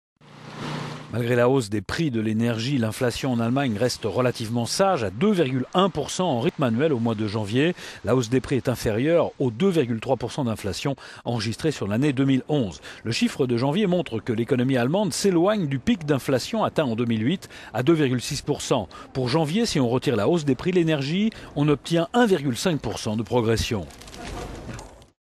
Inflation sage en Allemagne en janvier
Malgré la hausse des prix de l'énergie, l'inflation en Allemagne reste relativement sage : à 2,1% en rythme annuel au mois de janvier, la hausse des prix est inférieure aux 2,3% d'inflation enregistrée sur l'année 2011. Le chiffre de janvier montre que l'économie allemande s'éloigne du pic d'inflation atteint en 2008 à 2,6%. Pour janvier, si on retire la hausse des prix de l'énergie on obtient non plus 2,1% mais 1,5% de progression.